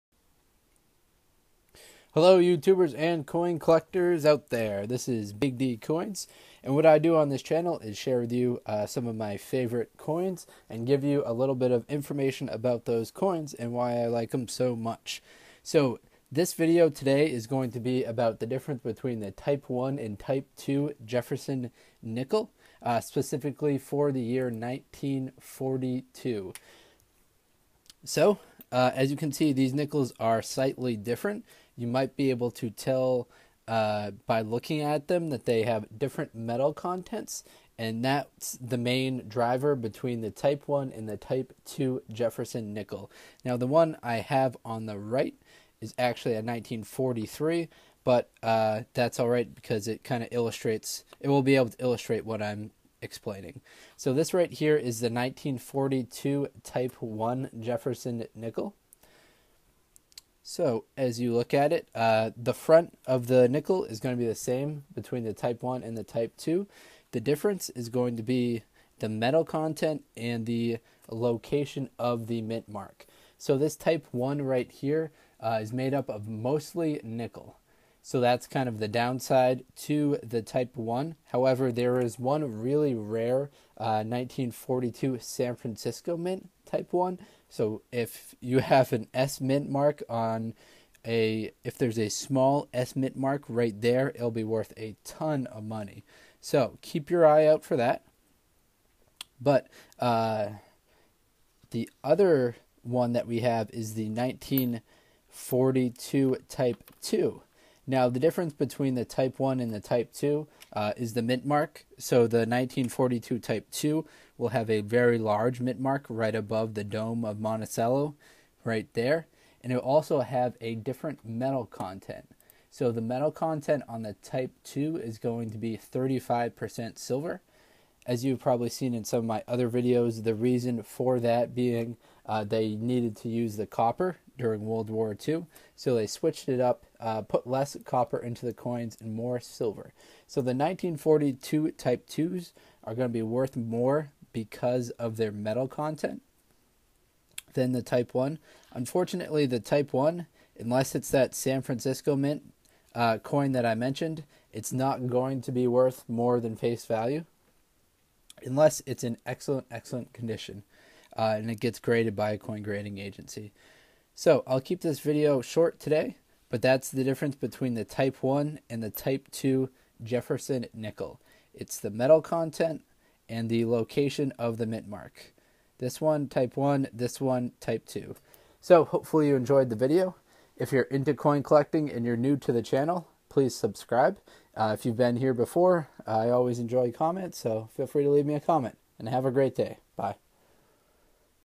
1942 Type 1 versus Type 2 Jefferson Nickel
If you are wondering what the difference is between a Type 1 and Type 2, 1942 P Jefferson Nickel and how to tell the difference in them.  Check the back (reverse) of the nickel and if there is a large P above the dome of the Jefferson Memorial then it is a Type 2 1942 P Jefferson Nickel.  If there is no mintmark on the coin at all it is a Type 1.  The difference is in the composite metals in the coin that was changed during the U.S. involvement in World War 2.  The Type 2 contains approx. 35% Silver to replace Nickel because Nickel was used to make weapons and armor during the WW2.